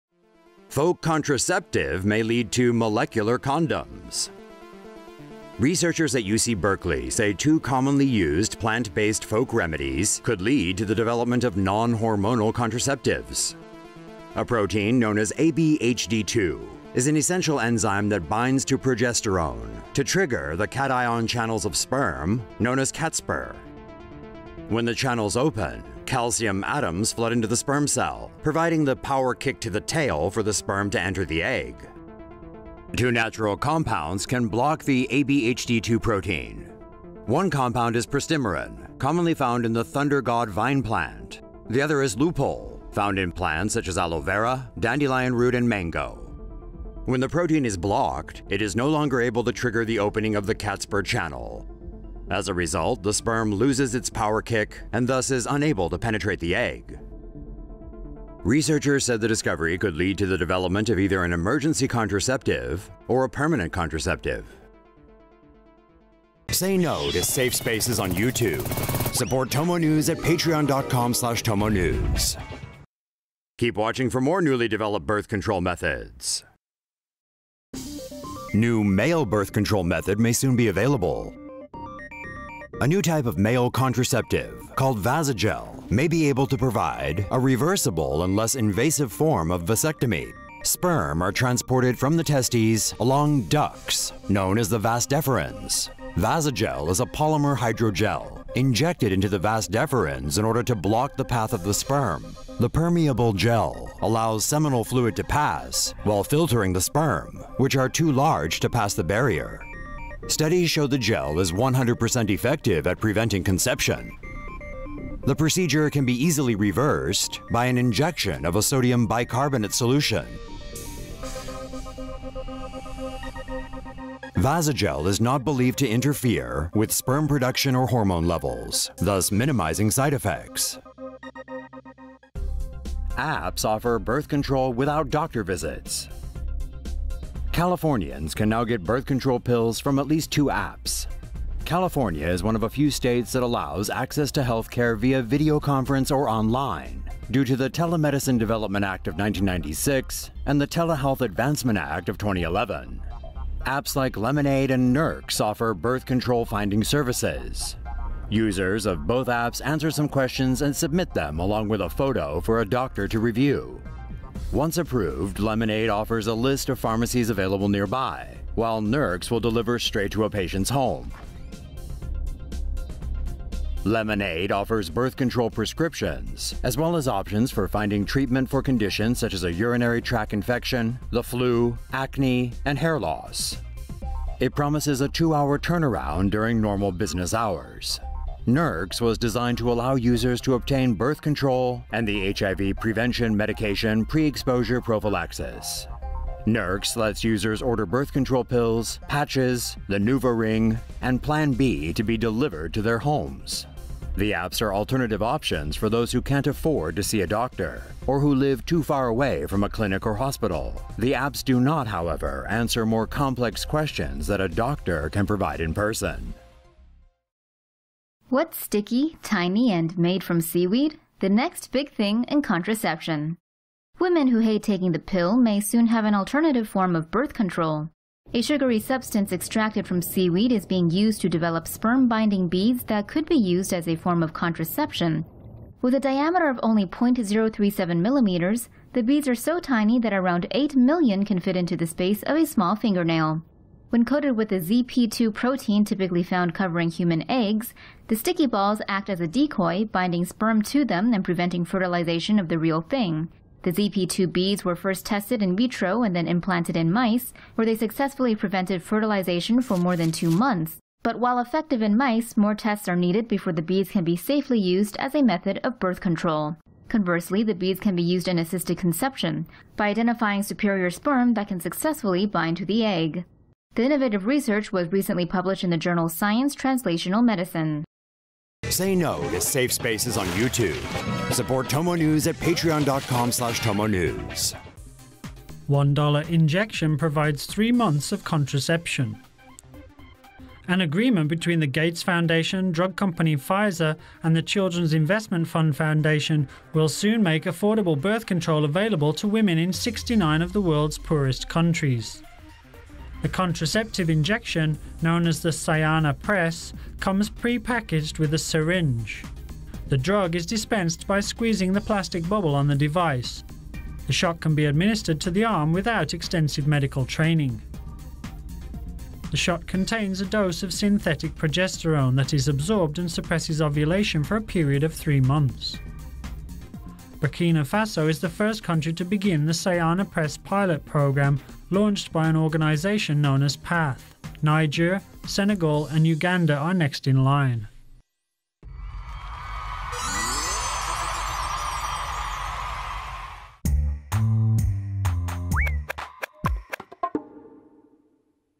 Anti-fertility folk medicine may be the future contraceptive - TomoNews
BERKELEY, CALIFORNIA — Researchers at UC Berkeley said two commonly used plant-based folk remedies could lead to the development of non-hormonal contraceptives.

The two natural compounds can block the ABHD2 protein. One compound is pristimerin, commonly found in the ‘thunder god vine’ plant. The other is lupeol, found in plants such as aloe vera, dandelion root and mango. ABHD2 is an essential enzyme that binds to progesterone, triggering the cation channels in sperm to open and let calcium atoms flood into the cell, giving the power kick the sperm tail needs to enter the egg.

When the protein is blocked, it is no longer able to trigger the opening of the channel, which is also known as CatSper. As a result, the sperm loses its power kick and is thus unable to penetrate the egg.

“Because these two plant compounds block fertilization at very, very low concentrations — about 10 times lower than levels of levonorgestrel in Plan B — they could be a new generation of emergency contraceptive we nicknamed ‘molecular condoms,'” Polina Lishko, an assistant professor of molecular and cell biology at UC Berkeley said in a press release.

Researchers said the discovery could lead to the development of either an emergency contraceptive or a permanent contraceptive.